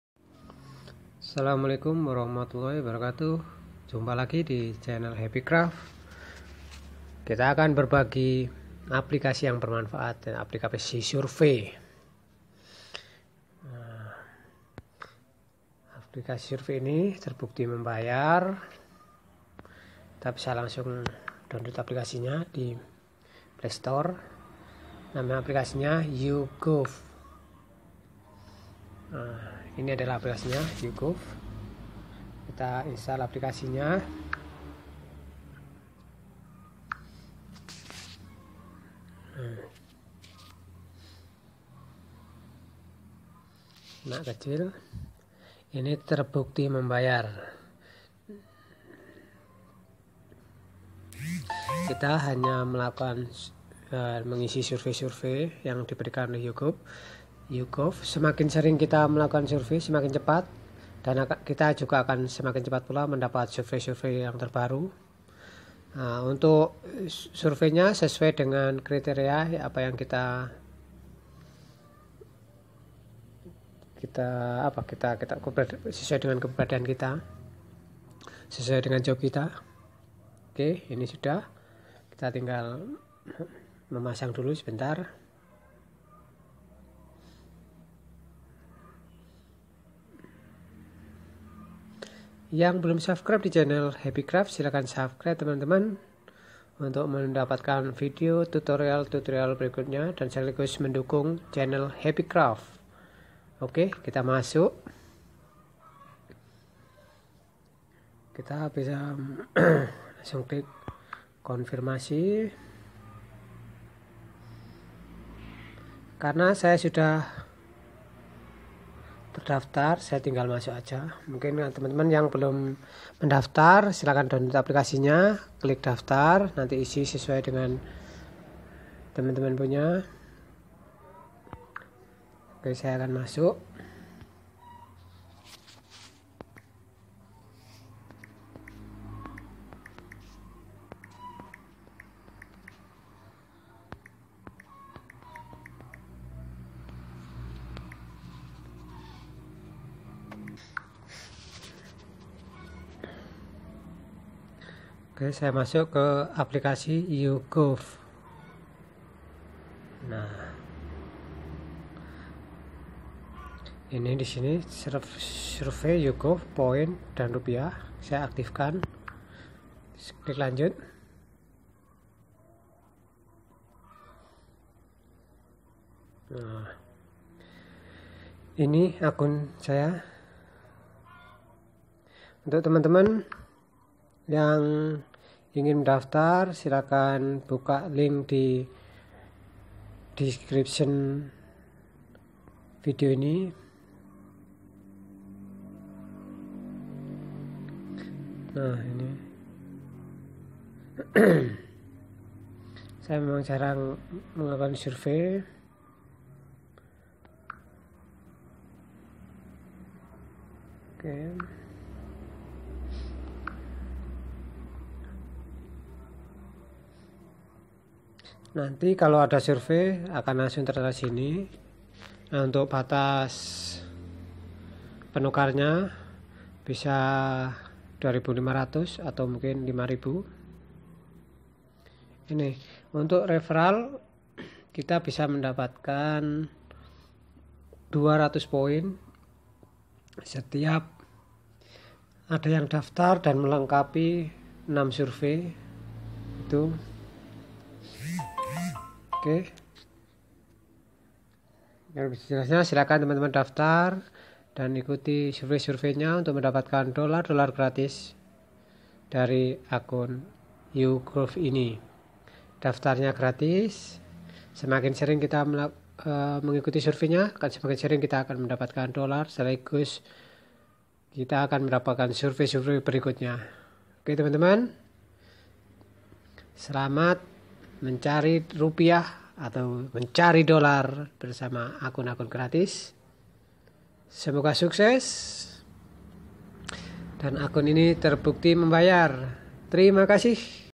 Cari uang gratis, ikut survei gratis
Cari uang gratis, ikut survei gratis melalui online. Survei ini daftarnya gratis alias tidak bayar. Setiap kali kita menyelesaikan survei kita dapat poin yang nantinya bisa kita tukarkan dollar gratis.
Untuk daftar silahkan ikuti link ini
Semakin kita aktip ikut survei semakin sering kita dapat survei survei baru dan automatis semakin cepat poin kita terkumpul